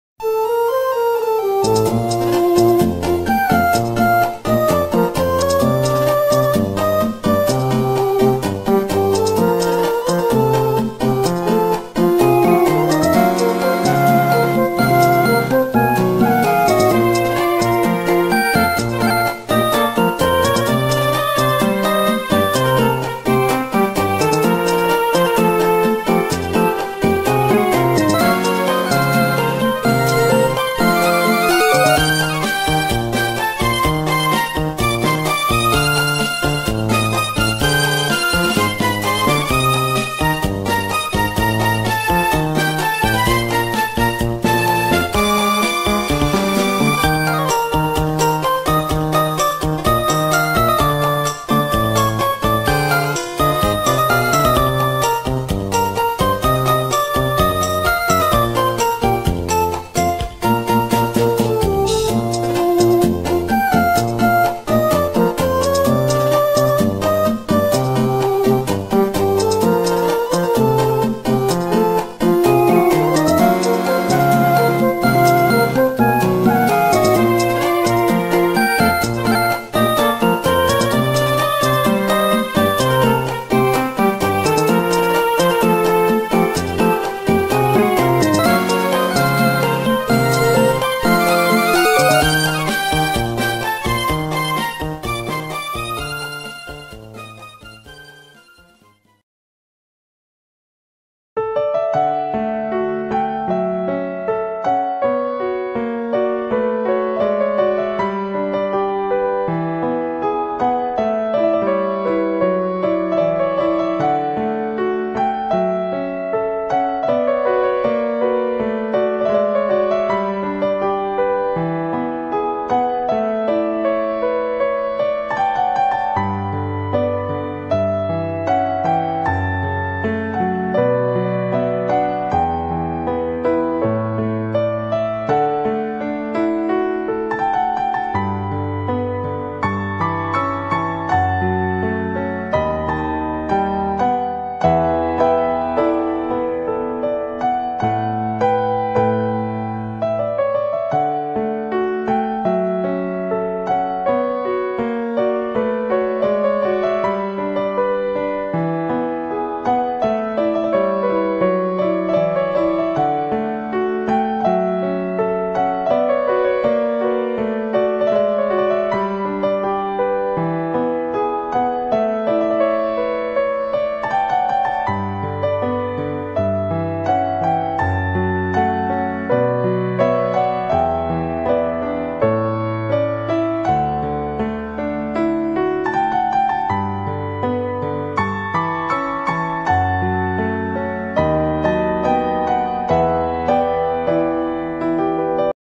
furret speedpaint
yo, new video

hope y'all like this QwQ
comment some pokémon for me to draw if ya want